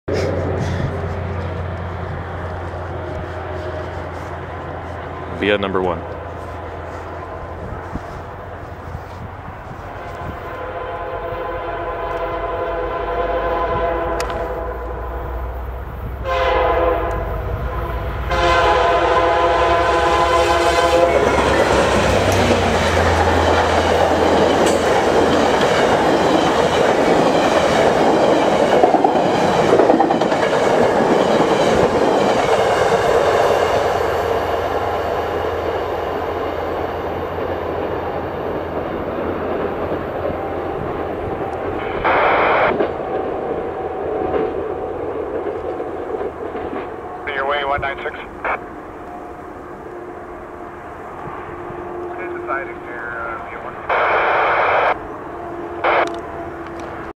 CN Train Spotting: VIA Sisters!!! | VIA 6426 & 6425 Leading VIA #1 Westbound 2/9 3/30/12
Here's my 16th total catch with Wheels069. After waiting awhile after the CWR train passed clogging the line, VIA came up following him a few miles behind. Here you see the Canadian (VIA #1) head westbound at Gainford AB. This train was going so fast we couldn't capture the numbers so we checked our videos afterwards and paused to see and there we had it, VIA 6426 leading and VIA 6425 trailing. What a good catch considering there sisters. This is 2/9 trains on March 30, 2012. Shot 9:53AM.

VIA 6426 (F40PH-2)
VIA 6425 (F40PH-2)

3/30/12